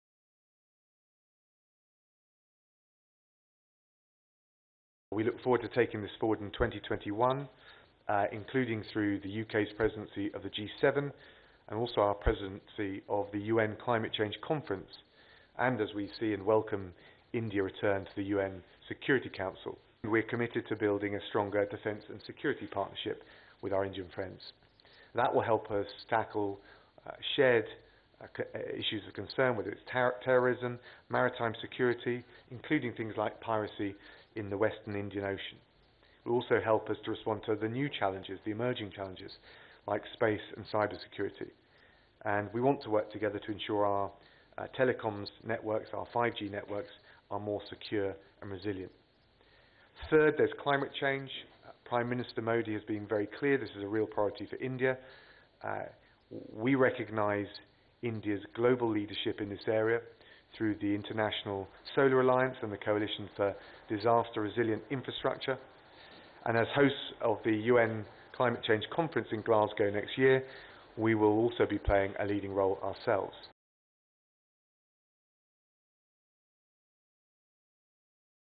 Committed to build stronger defence with India to tackle terrorism: UK Foreign Secy
New Delhi, Dec 15 (ANI): While addressing the media in the national capital, UK Foreign Secretary Dominic Raab said, “We look forward to taking the spot in 2021 including the UK's presidency of G7 and also our presidency of UN Climate Change Conference. We welcome India's return to UN Security Council”. He further added that UK wants to deepen their economic partnership with India. “We're committed to building a stronger defence and security partnership with India, that will help us tackle shared issues related to terrorism, maritime security including piracy in Western Indian ocean”.